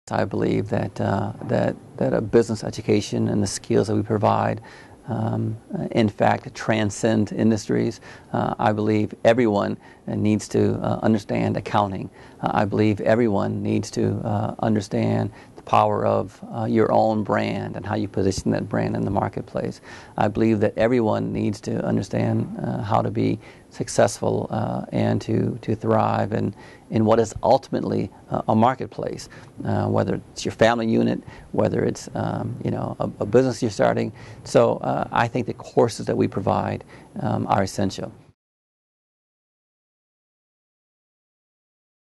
Dean E. LaBrent Chrite - Business Education
Dean E. LaBrent Chrite of the Feliciano School of Business talks about the importance of business education.